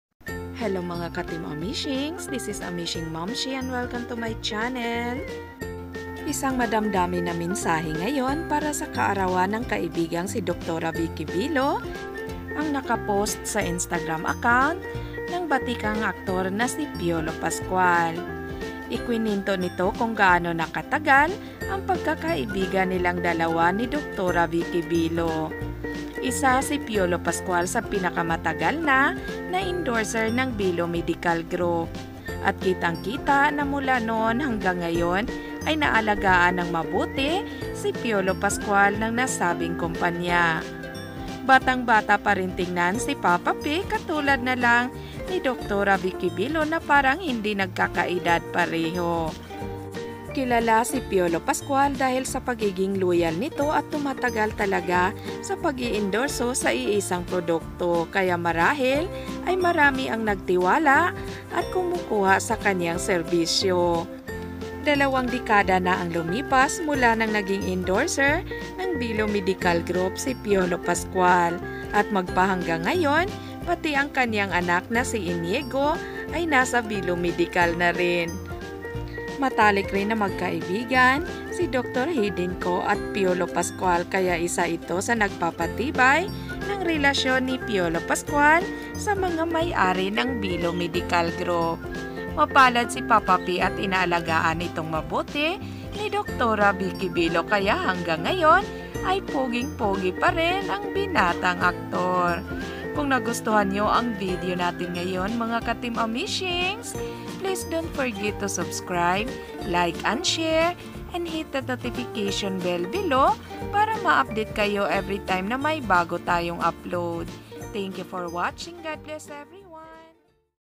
PIOLO PASCUAL MAY REBELASYON PATUNGKOL KAY DRA. VICKY BELO!
This vlog is about piolo pascual and dra. Vicky belo!